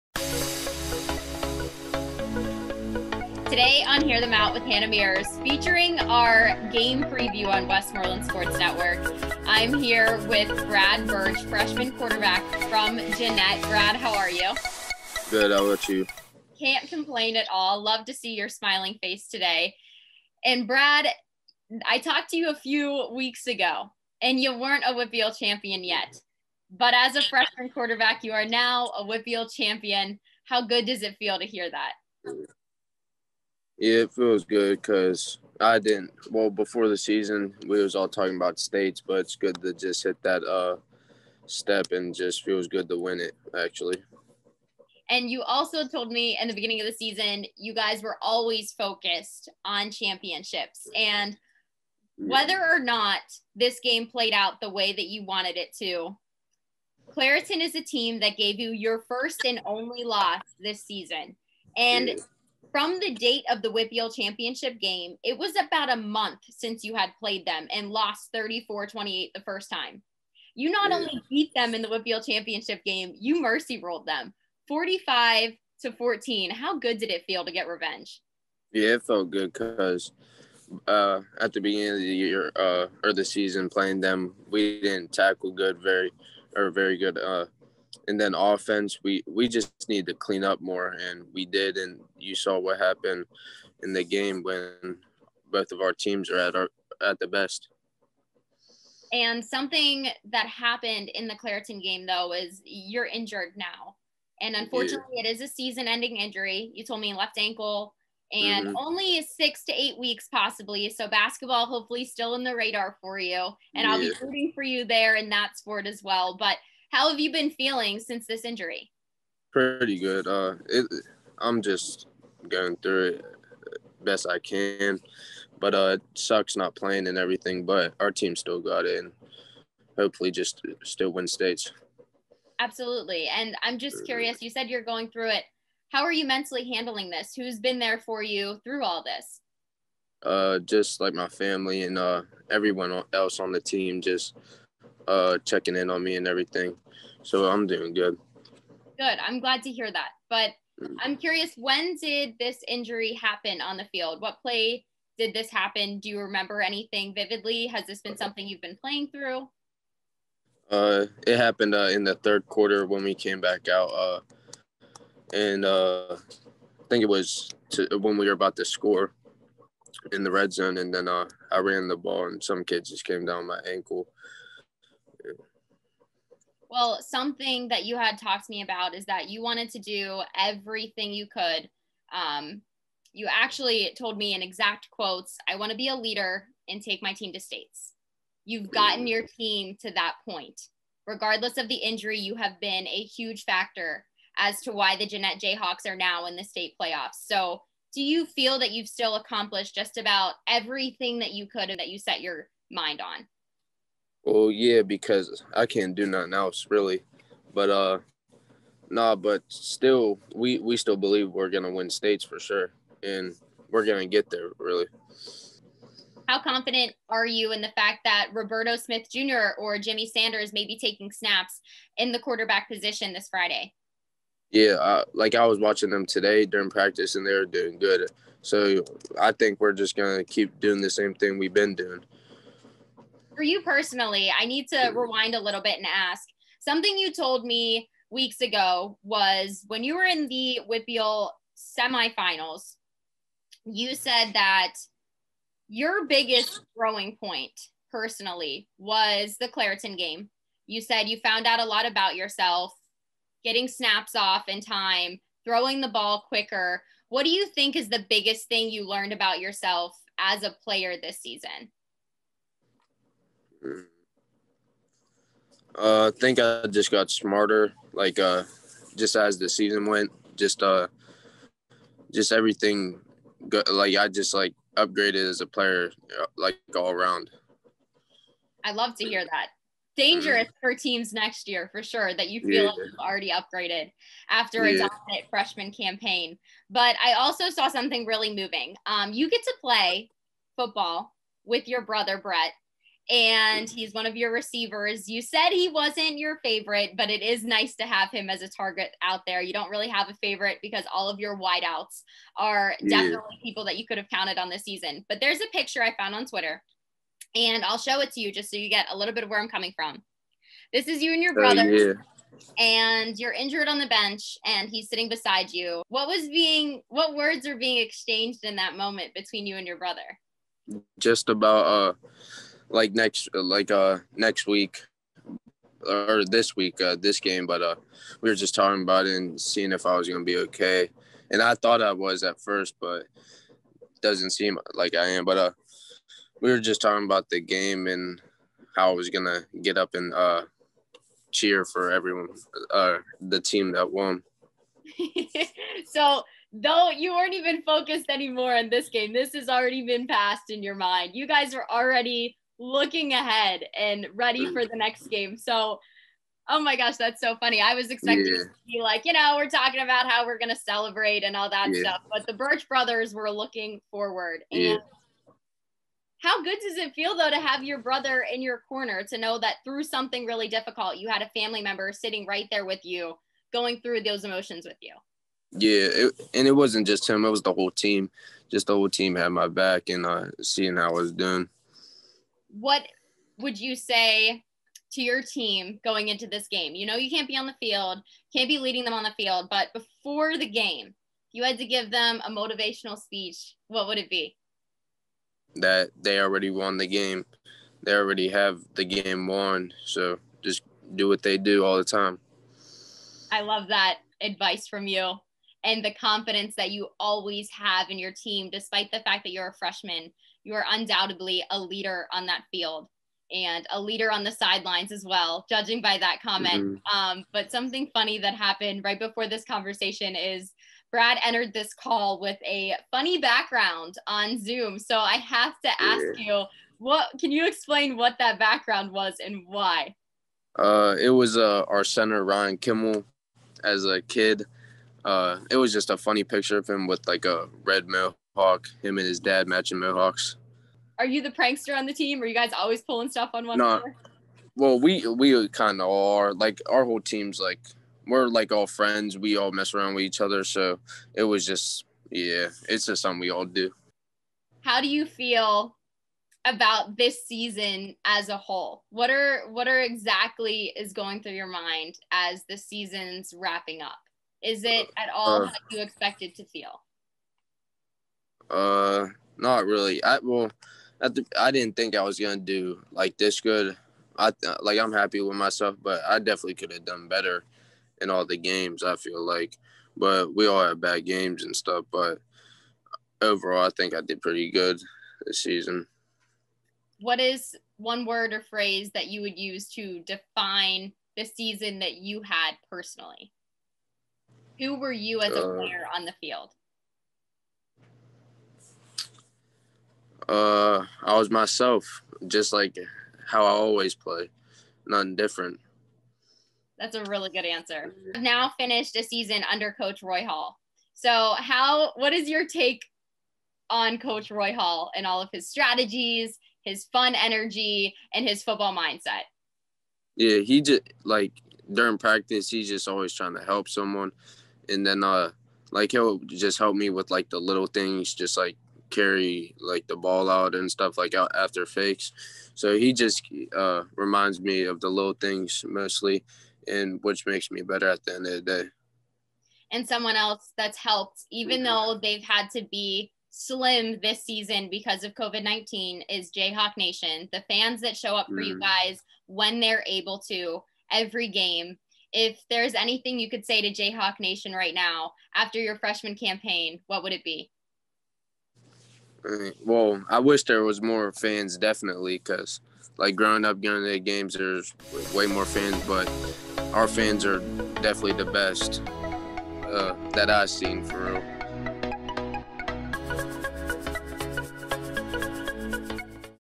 Season Summary For Real: Brad Birch
Jeannette's Freshman QB, Brad Birch, accomplished his goal of leading his team to a WPIAL title but suffered a season ending, ankle injury in the process. Birch talks the title, the injury, his team moving forward and answers rapid fire questions!